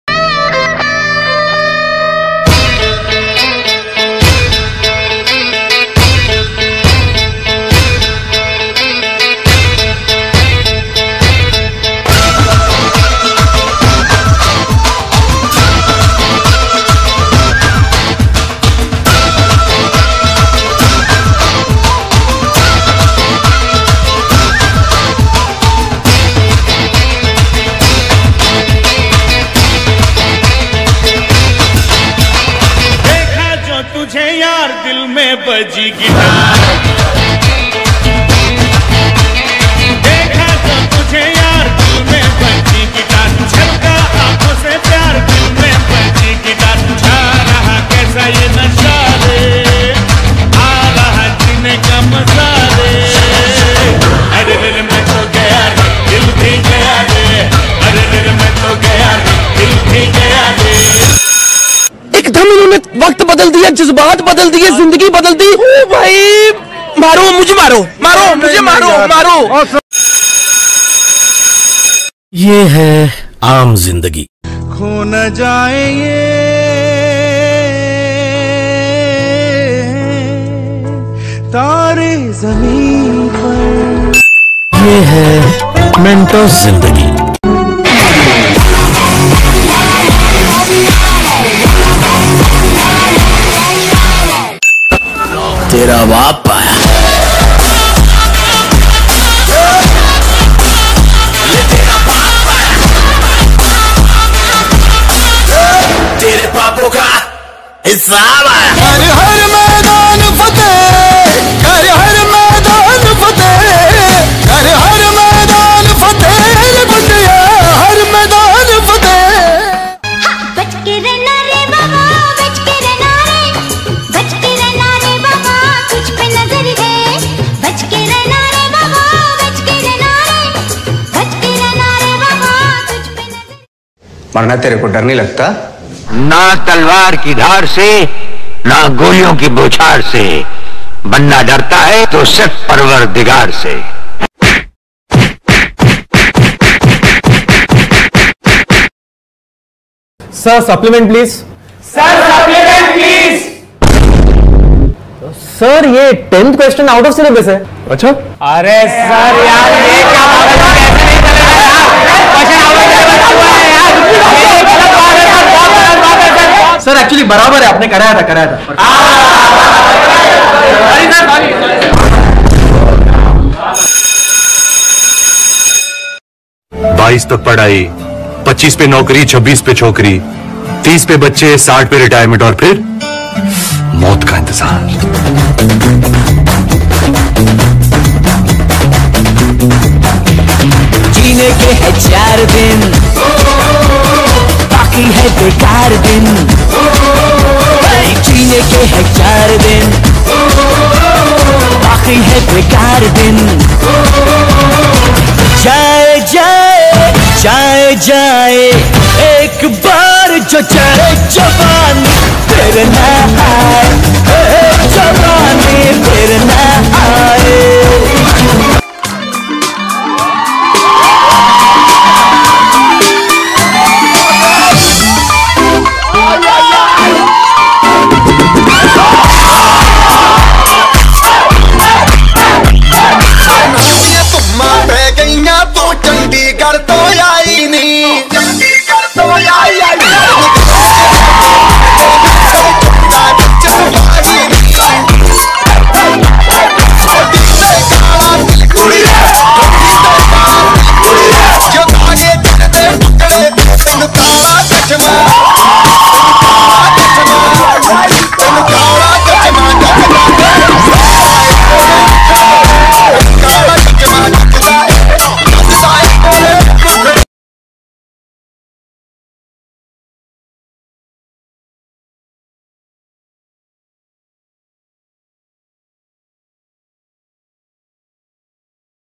Boys Dance | Hindi Bollywood songs | Best group performance | Annual Function 2022
SIR EINSTEIN ACADEMY’S Digital Teaching Assets